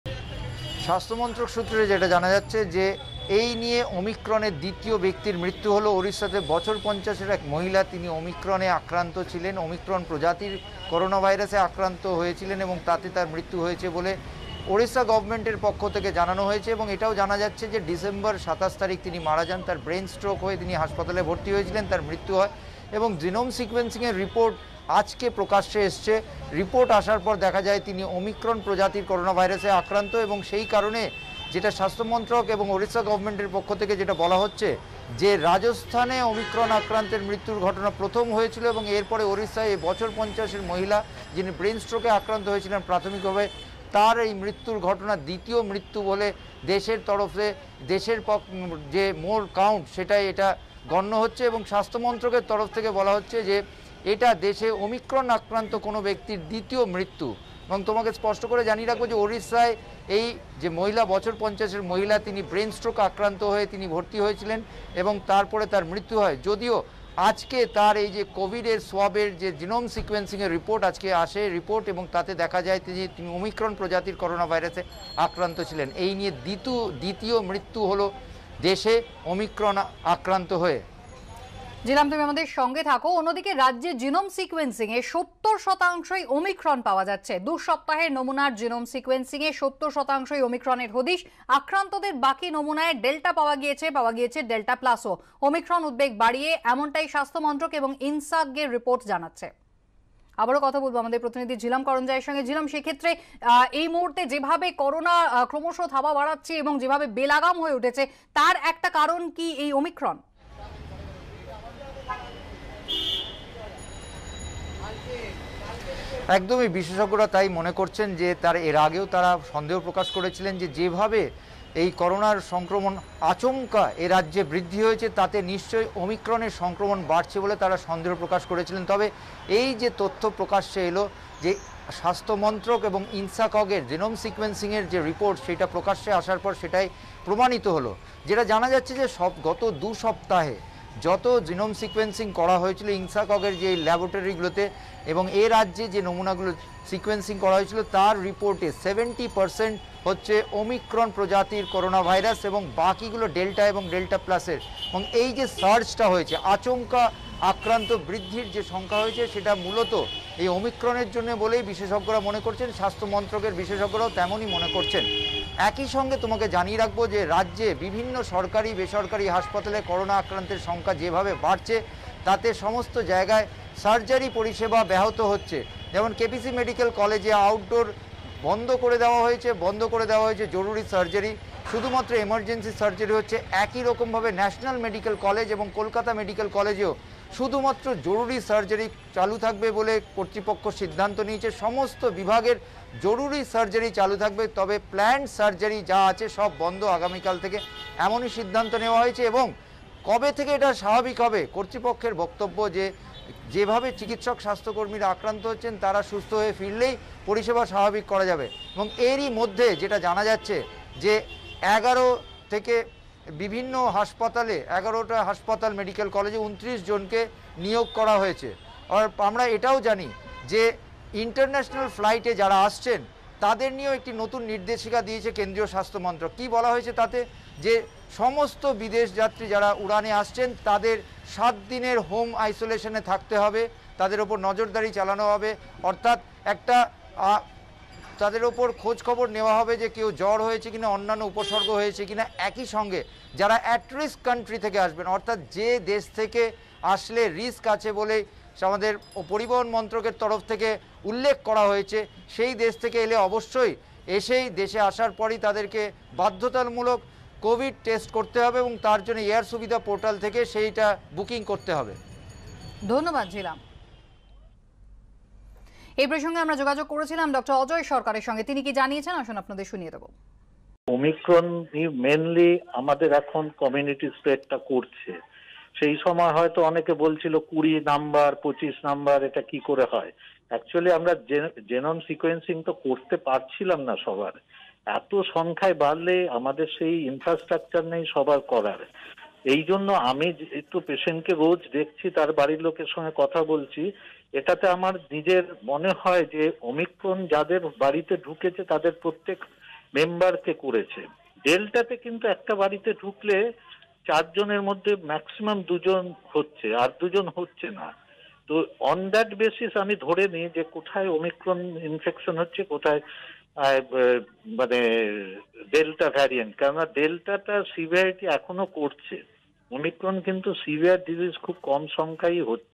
Omicron Death: রাজস্থানের পর ওড়িশা, ওমিক্রনে দেশে দ্বিতীয় মৃত্যু | Bangla News
রাজস্থানের (Rajasthan) পর ওমিক্রনে (Omicron) দেশে দ্বিতীয় মৃত্যু ওড়িশায় (Odisha)। বুরলা মেডিক্যালে ব্রেন স্ট্রোকে আক্রান্ত মহিলার ওমিক্রন। ২৭ ডিসেম্বর মৃত্যুর পরে ওমিক্রন রিপোর্ট পজিটিভ।